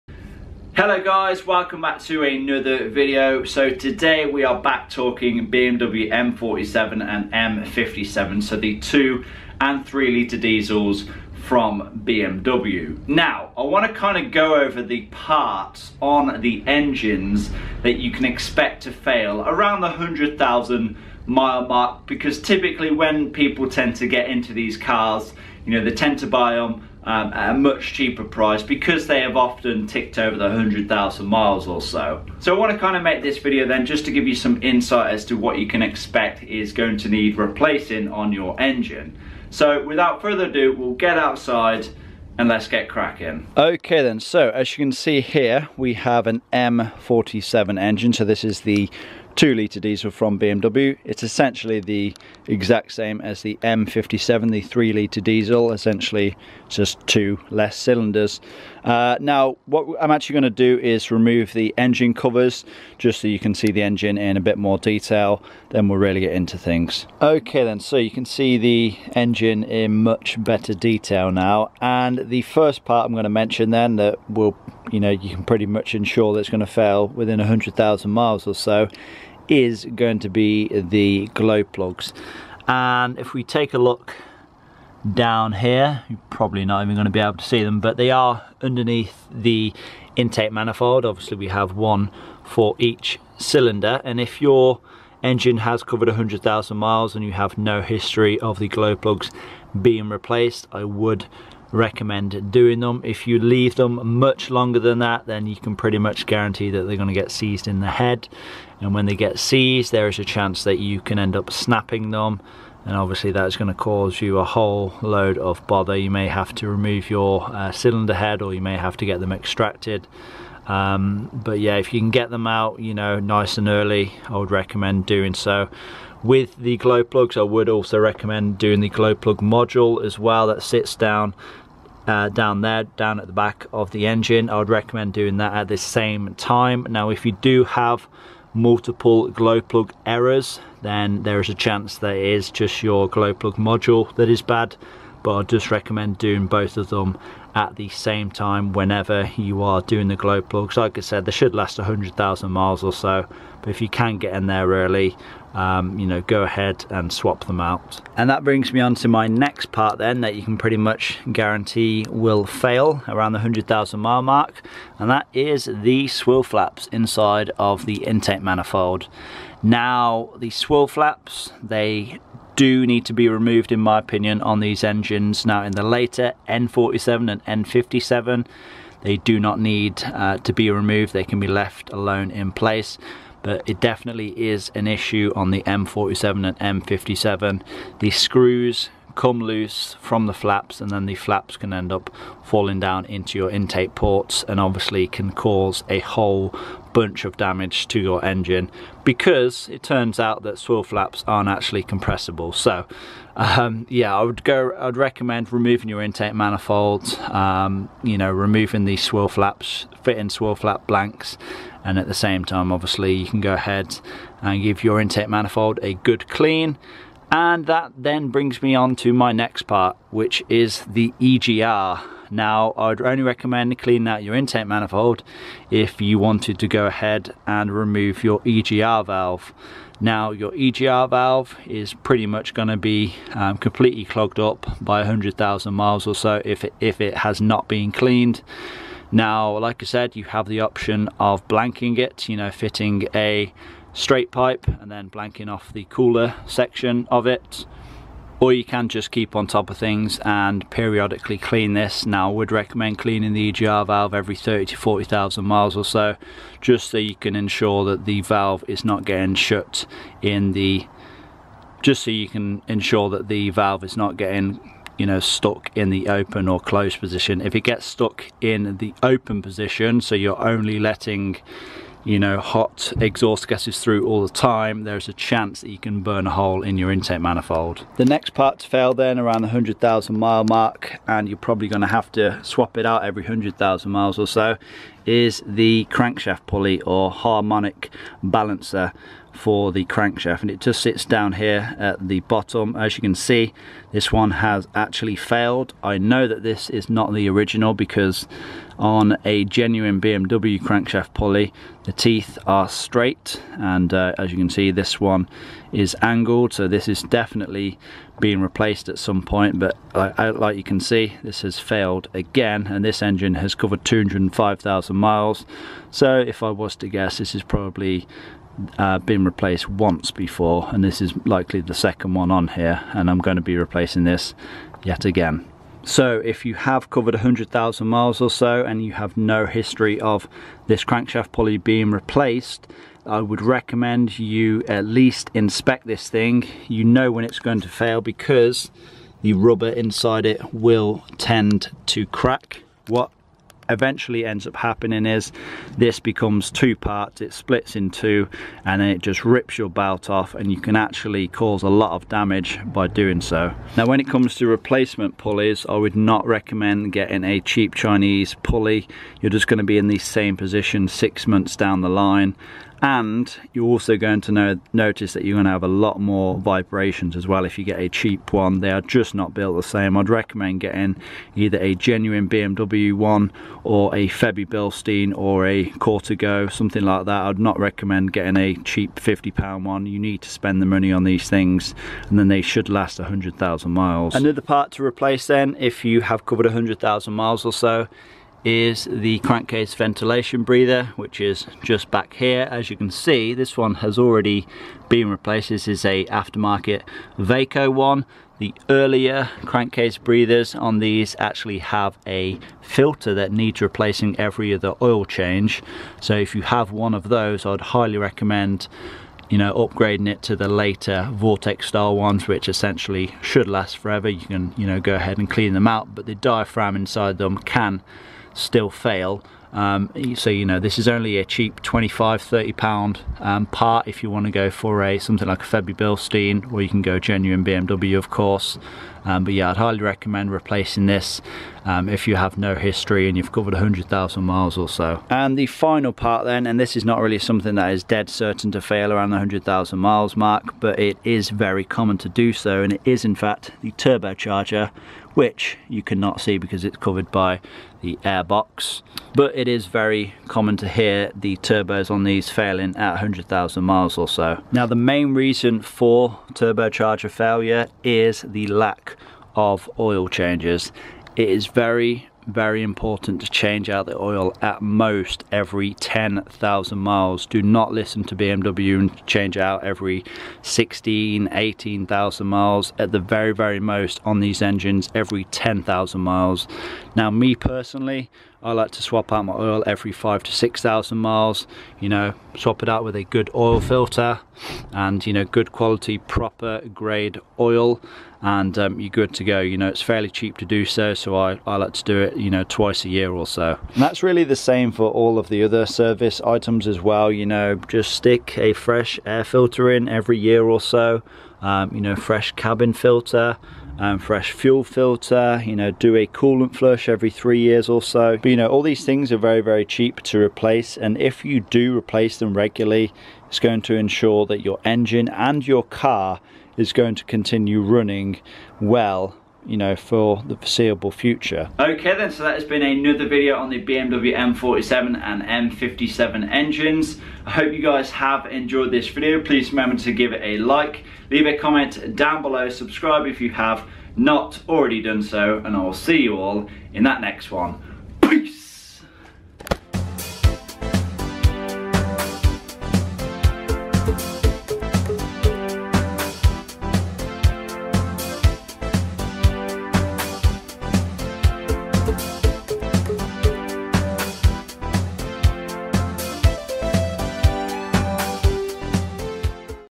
BMW M47 & M57 Engine Parts That Fail At 100,000 Miles
BMW M47 and BMW M57 engine parts that fail at 100,000 miles.

Small Impact Wrench
Big Impact Wrench
Sealey Impact Socket Set
Torx Socket Set
Female Torx Socket Set
Torque Wrench